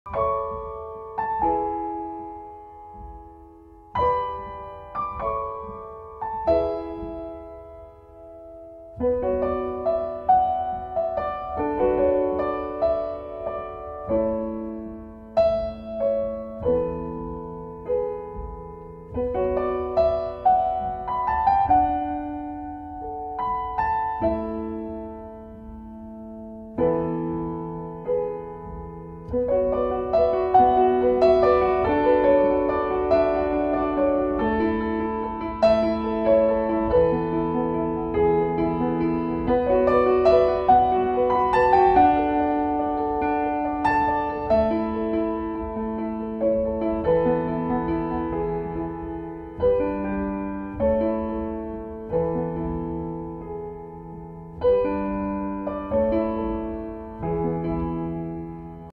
Urban sketch of a rustic door
Today i drew this urban sketch of a rustic door , i used .003 .005 and  .02 micron pens on a stillman and birn zeta series
Sketch book